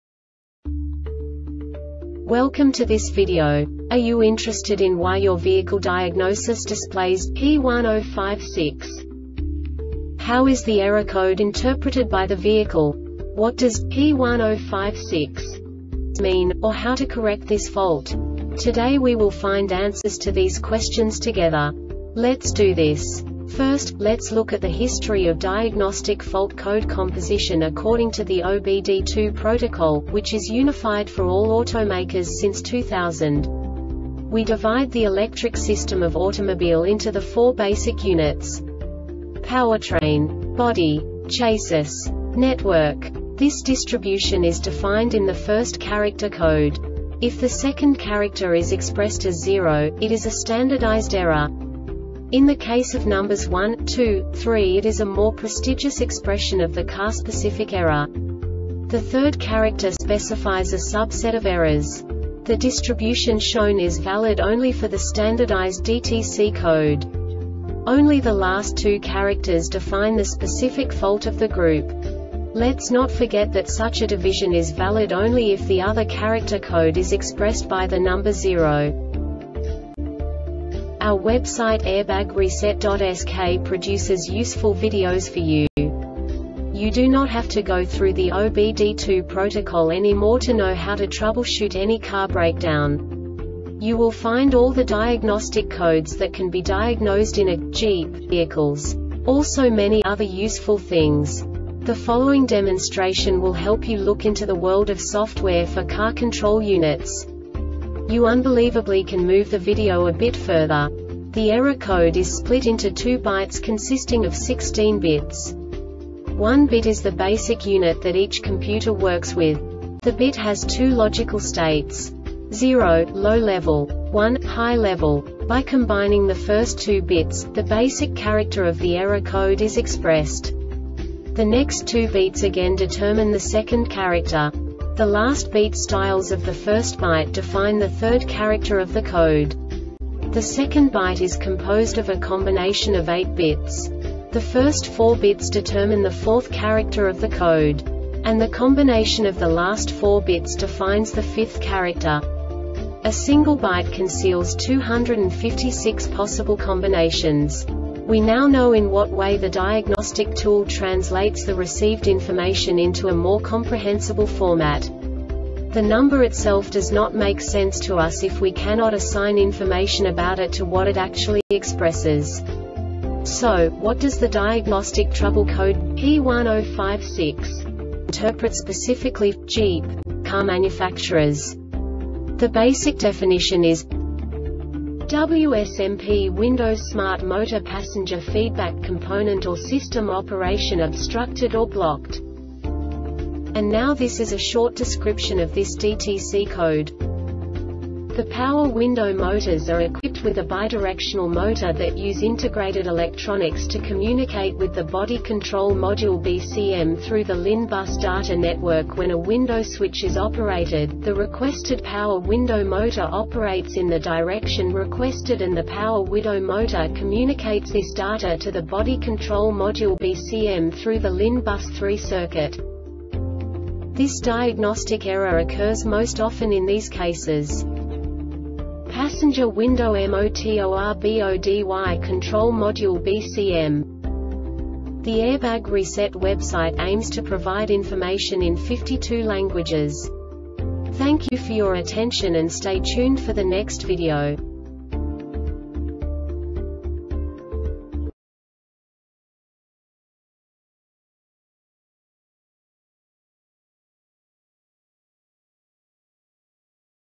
DTC Jeep P1056 Short Explanation
Contents:
0:21 Basic DTC analysis according to OBD2 protocol standard.
1:48 Insight into programming
2:58 A diagnostically understandable description of the Diagnostic Trouble Code
3:05 Basic error definition

Make:
Jeep
Code:
P1056
Definition:
Wsmp (Window Smart Motor Passenger) Feedback-Component Or System Operation Obstructed Or Blocked
Description:
The Power Window Motors are equipped with a bidirectional motor that use integrated electronics to communicate with the Body Control Module (BCM) through the LIN Bus data network. When a Window Switch is operated, the requested Power Window Motor operates in the direction requested and the Power Widow Motor communicates this data to the Body Control Module (BCM) through the LIN Bus 3 circuit.
Cause:
PASSENGER WINDOW MOTORBODY CONTROL MODULE (BCM)